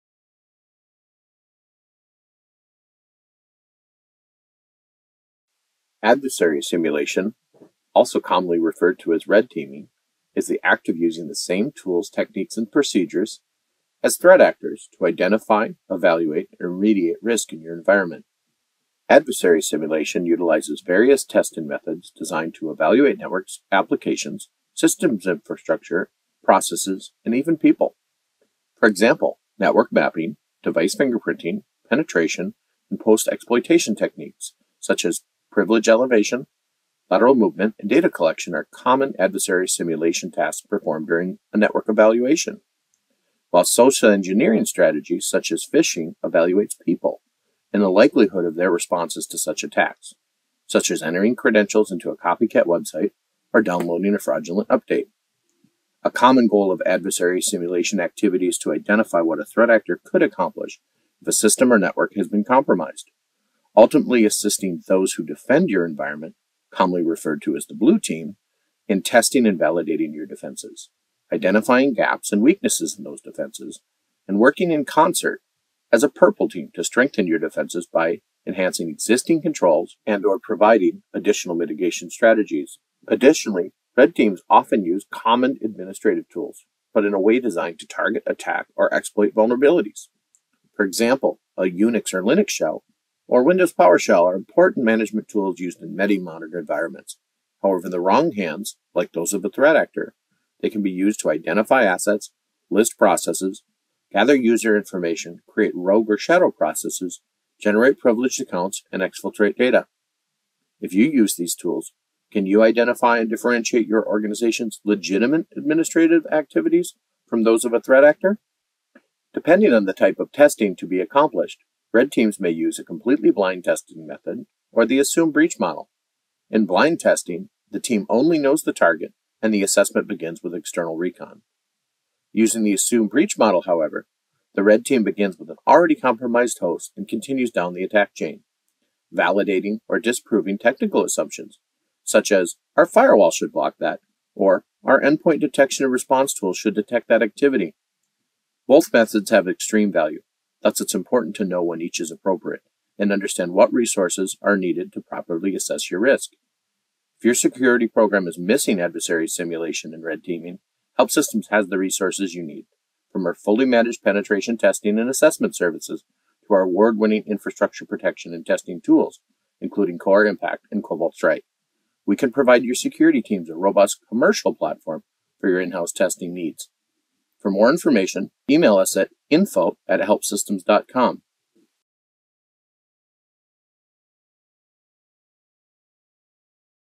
What is Adversary Simulation?
This short video reveals what adversary simulation is, why it's important to conduct them, and how solutions can help you conduct them more efficiently.